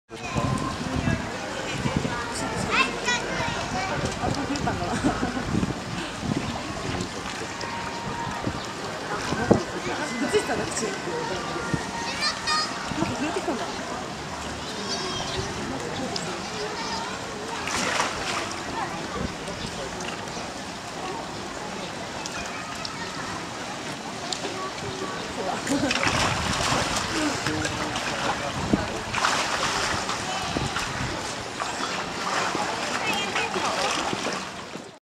東山動物園「アシカ親子の毛繕い」
東山動物園のカリフォルニアアシカ親子です。
ここではよく子供が生まれています。
岩の上での授乳シーンなども見られますよ。

（10/02/14(日)にzoomeで公開 zoome閉鎖につき移転）